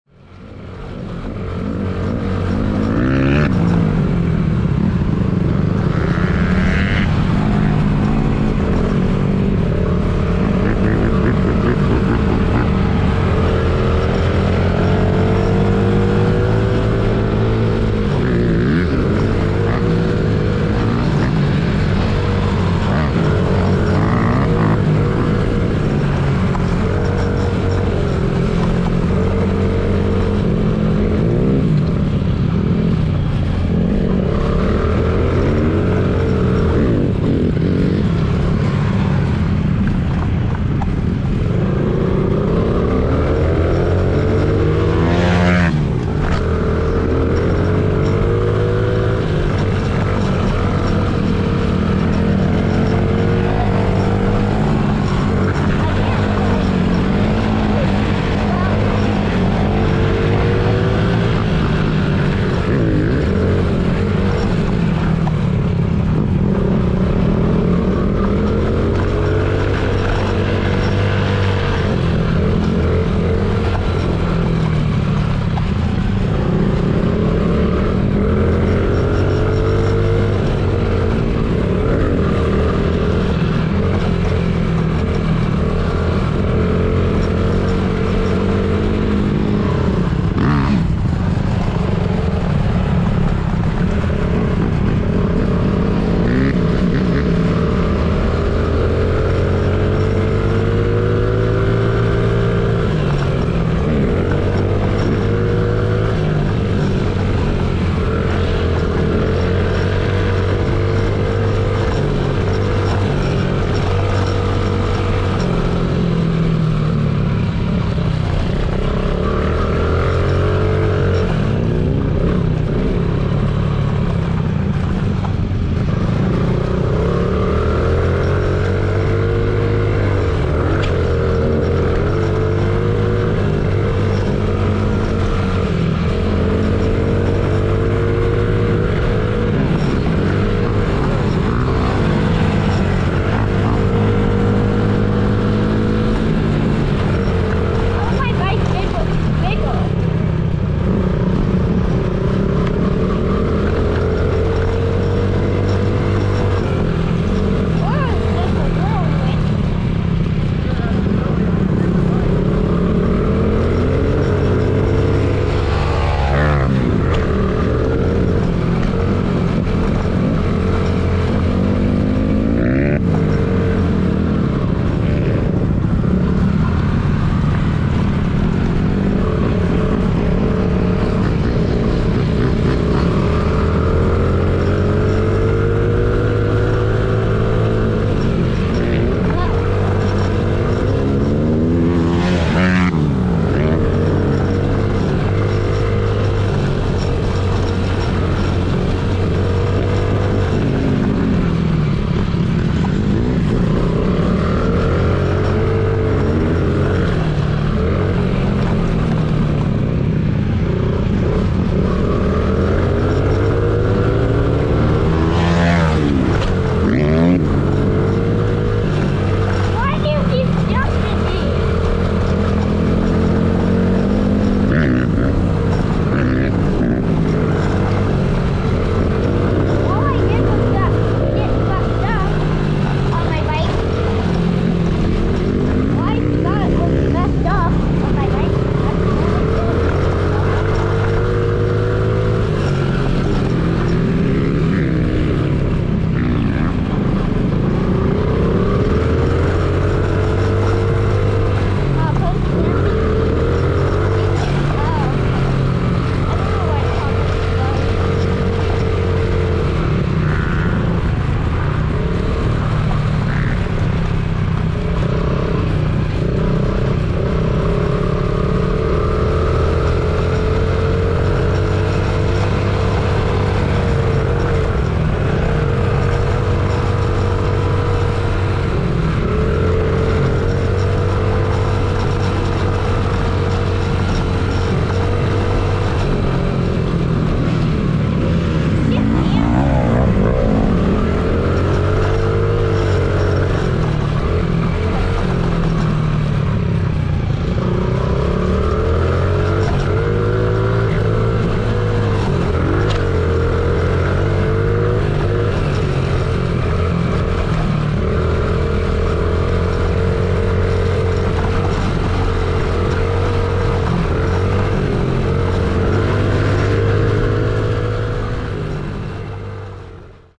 Is She Finding Her Speed 🤔 | Raw GoPro Clip 2023 | AOT Racing ✊😁
This is some Go Pro footage of Brianna on her 2020 Honda CRF50 during open practice out on the AM track at Airway X. Huge shout out to @AOTRacing for helping Brianna24 look her best while she's out there working hard to try and keep up with the faster kids while also helping to build confidence in those with less experience 👊😎